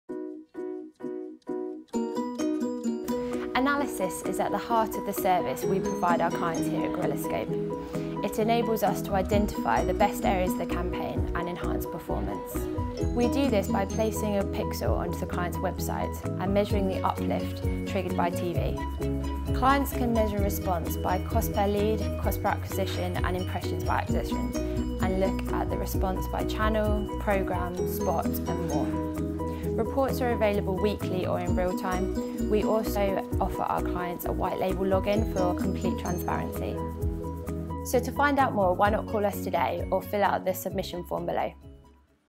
Campaign Optimisation | Guerillascope
How the Guerillascope team uses powerful cross-platform analysis software to lower Cost Per Lead, Cost Per Acquisition and more to enhance campaign efficiency.